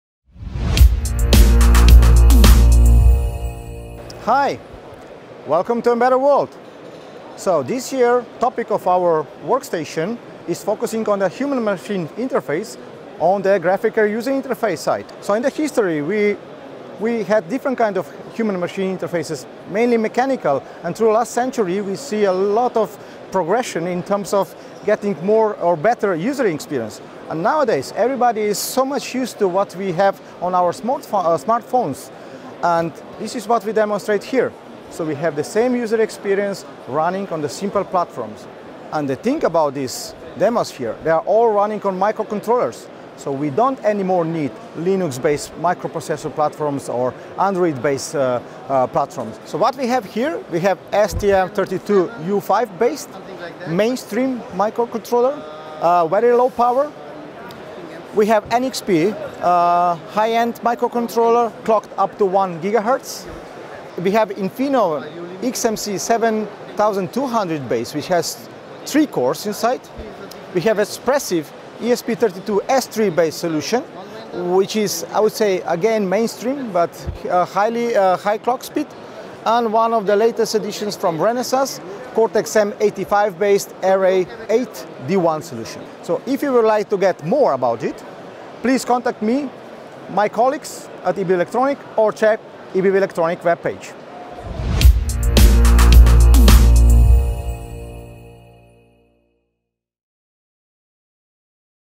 HMI - Human Machine Interfaces - EBV at Embedded World 2024
Learn more about the paradigm Shift: MCU's Evolution into Smartphone-Like Interfaces with Uros Mali - Director Technology Segments at EBV